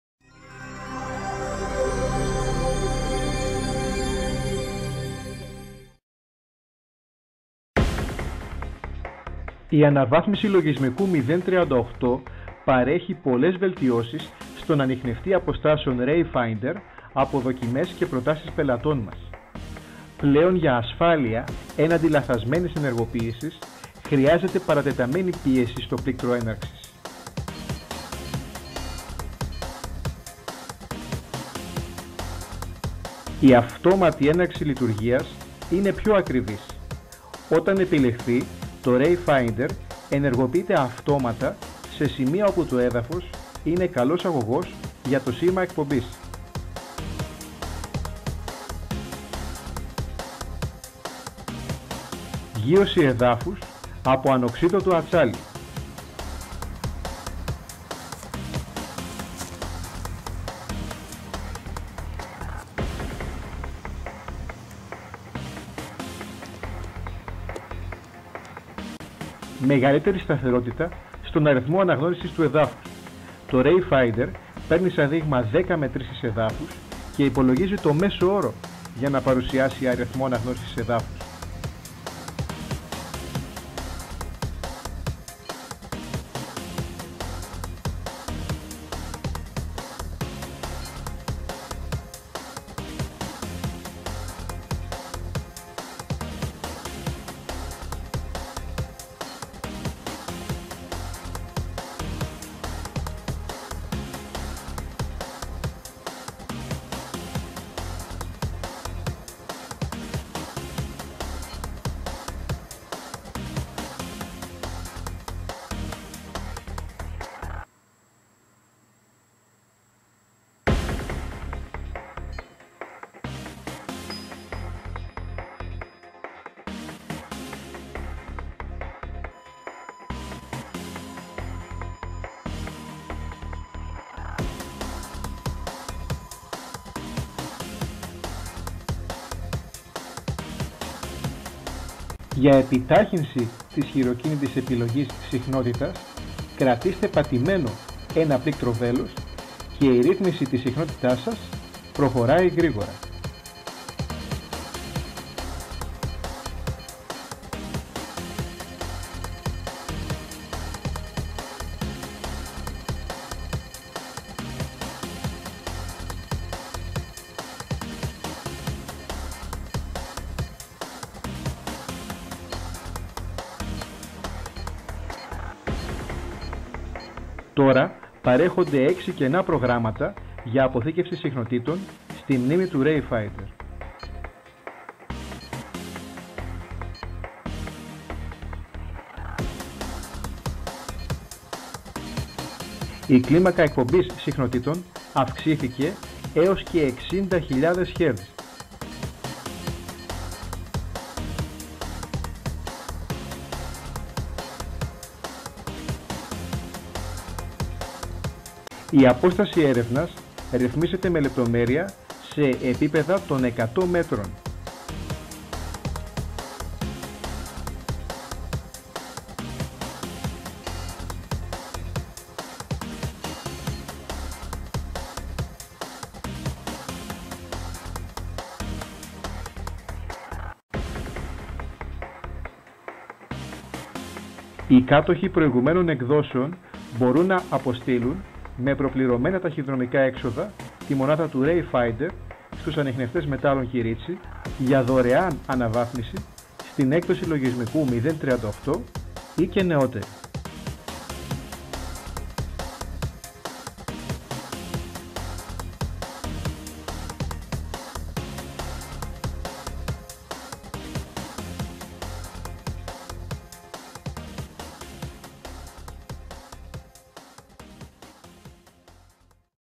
ανιχνευτές χρυσού αποστάσεως RAYFINDER δωρεάν αναβάθμιση
ανιχνευτές μετάλλων - χρυσού αποστάσεως Rayfinder δωρεάν αναβάθμιση λογισμικού απο Ανιχνευτές Μετάλλων Κυρίτσης. Ραβδοσκοπία, βέργες Γ εκκρεμές. Πληροφορίες